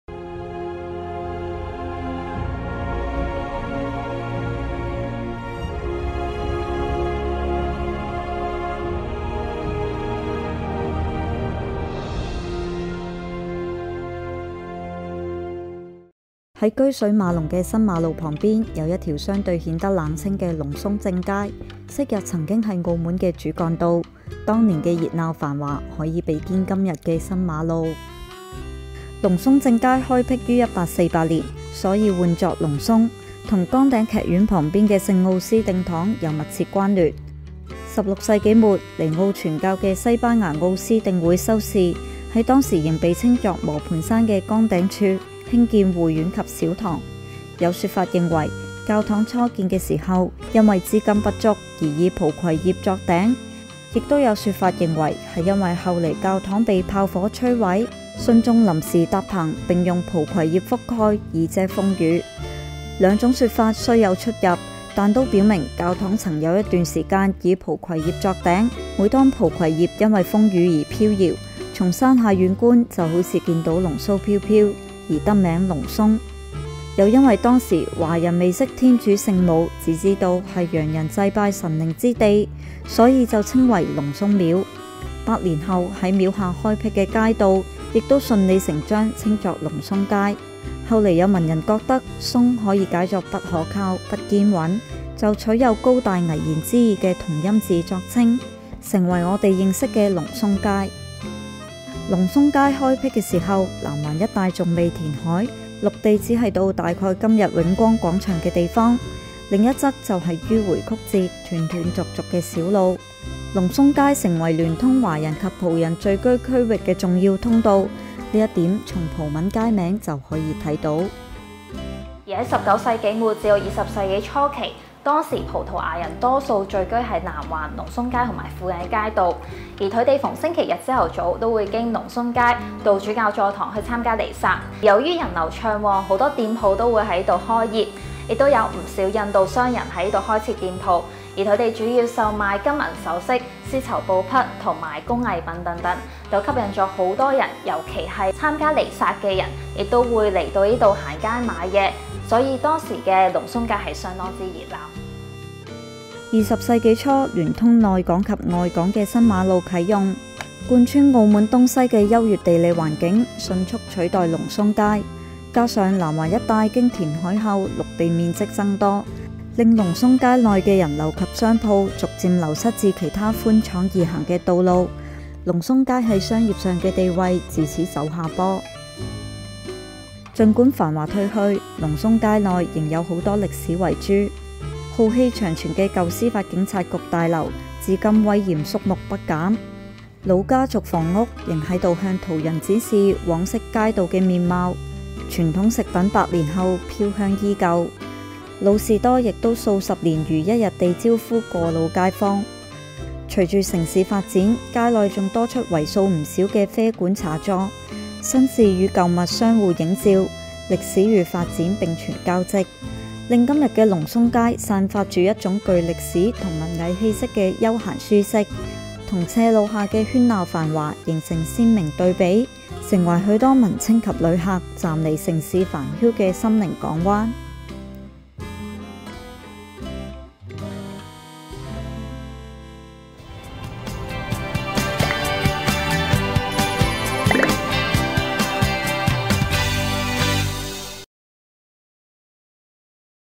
龍嵩正街 繁華褪去 餘韻仍在
【澳門記憶】在車水馬龍的新馬路旁，有一條相對顯得冷清的龍嵩正街，但這條開闢於1848年的街道曾是聯通澳門中葡社群的重要街道，原名亦不叫“龍嵩街”。